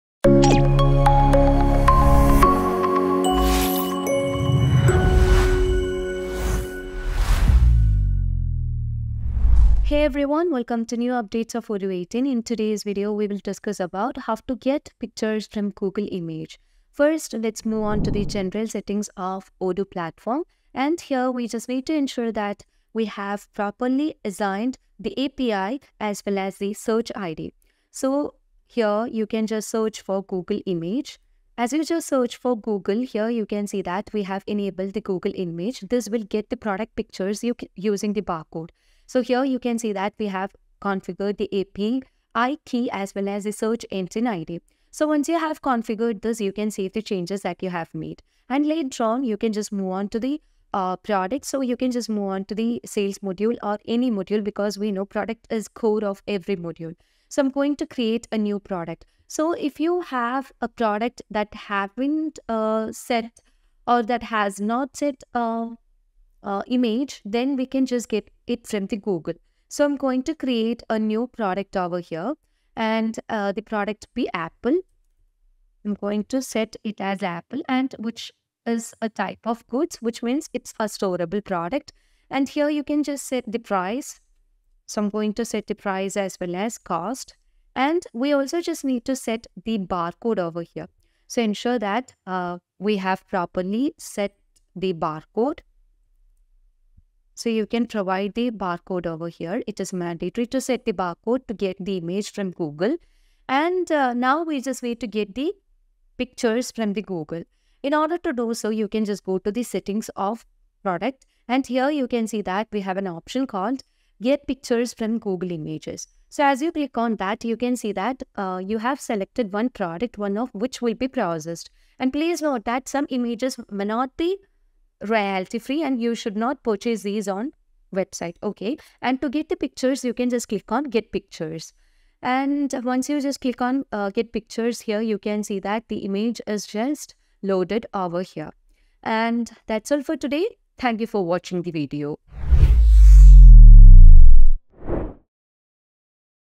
How to Get Pictures From Google Image in Odoo 18 | Odoo 18 Features | Odoo 18 Release Date
Having appropriate product images in Odoo is useful for a number of reasons. However, if a lot of products need images, assigning them can become incredibly time-consuming.

Fortunately, by configuring the Google Custom Search API within an Odoo database, finding product images for products (based on their barcode) is extremely efficient.

Configuration
In order to utilize Google Custom Search within an Odoo database, both the database and the Google API must be properly configured.

 Note

Free Google accounts allow users to select up to 100 free images per day. If a higher amount is needed, a billing upgrade is required.

Google API dashboard
Go to the Google Cloud Platform API & Services page to generate Google Custom Search API credentials. Then, log in with a Google account.

From here, select (or create) an API project to store the credentials. Start by giving it a memorable Project Name, select a Location (if any), then click Create.

With the Credentials option selected in the left sidebar, click Create Credentials, and select the API key from the drop-down menu. Doing so reveals an API key created pop-up window, containing a custom API key. Copy and save Your API key in the pop-up window – it will be used later. Once the key is copied (and saved for later use), click Close to remove the pop-up window.

On this page, search for Custom Search API, and select it. From the Custom Search API page, enable the API by clicking Enable.

Google Programmable Search Dashboard
Next, go to Google Programmable Search Engine, and click either of the Get Started buttons. Log in with a Google account, if not already logged in. On the Create a new search engine form, fill out the name of the search engine, along with what the engine should search, and be sure to enable Image Search and SafeSearch.

Validate the form by clicking Create.

Doing so reveals a new page with the heading: Your new search engine has been created. From this page, click Customize to open the Overview ‣ Basic page. Then, copy the ID in the Search engine ID field. This ID is needed for the Odoo configuration.

Odoo
In the Odoo database, go to the Settings app and scroll to the Integrations section. From here, check the box beside Google Images. Then, click Save. Next, return to the Settings app, and scroll to the Integrations section. Then, enter the API Key and Search Engine ID in the fields beneath the Google Images feature.

Click Save.

Product images in Odoo with Google Custom Search API
Adding images to products in Odoo can be done on any product or product variant. This process can be completed in any Odoo application that provides access to product pages (e.g. Sales app, Inventory app, etc.).

Below is a step-by-step guide detailing how to utilize the Google Custom Search API to assign images to products in Odoo using the Odoo Sales application:

Navigate to the Products page in the Sales app (Sales app ‣ Products ‣ Products). Or, navigate to the Product Variants page in the Sales app (Sales app ‣ Products ‣ Product Variants).

Select the desired product that needs an image.

 Note

Only products (or product variants) that have a barcode, but not an image, are processed.

If a product with one or more variants is selected, each variant that matches the criteria above is processed.

Click the Action ⚙️ (gear) icon on the product page, and select Get Pictures from Google Images from the menu that pops up. On the pop-up window that appears, click Get Pictures.

Once clicked, the image(s) will appear incrementally.